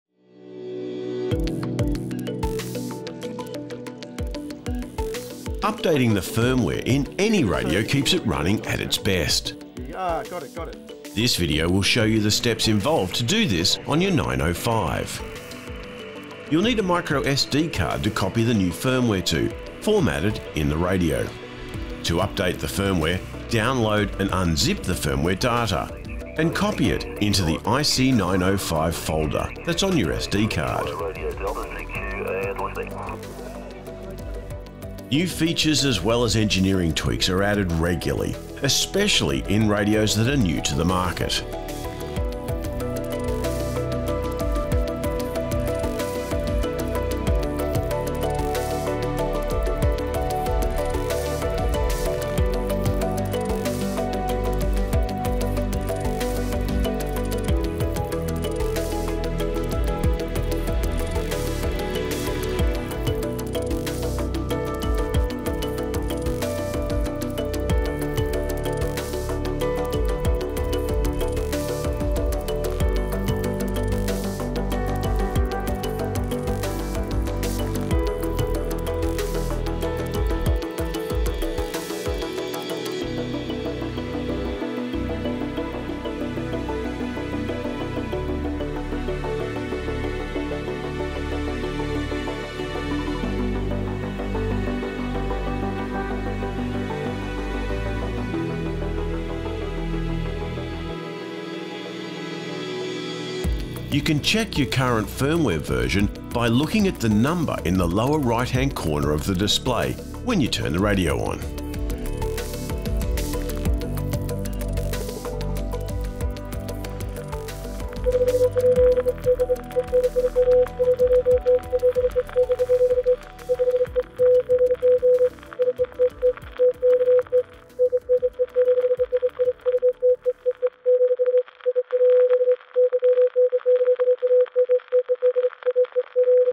How to update the Icom IC 905 Firmware
Updating the firmware of any radio keeps it running at its best and the IC-905 is no different. New features as well as engineering tweaks are added regularly, especially in radios that are new to the market.

This video will show you how easy it is to update the firmware in your radio. You’ll need a micro SD card formatted in the 905 to copy the new firmware to.

For context this video is part of an IC-905 review located